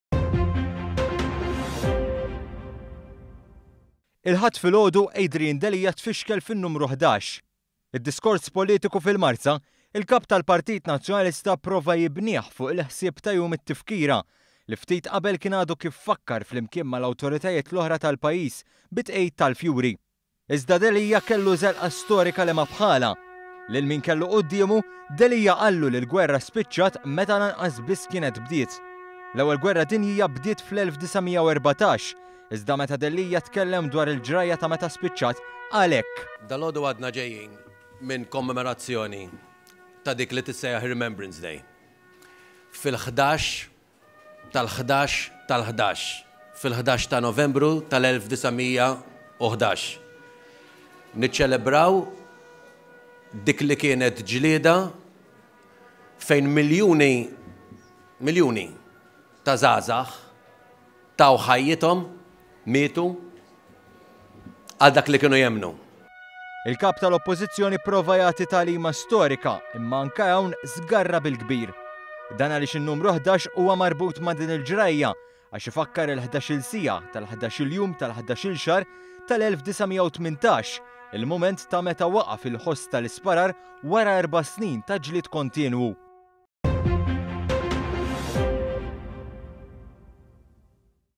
DELIA JITGERFEX FIN-NUMRU ĦDAX U L-ISTORJA
ONE.COM.MT - Fuq nota differenti, il-Kap tal-Oppozizzjoni Adrian Delia tfixkel bil-kbir fin-numru ħdax meta prova jagħti tagħlima storika fuq Jum it-Tifkira.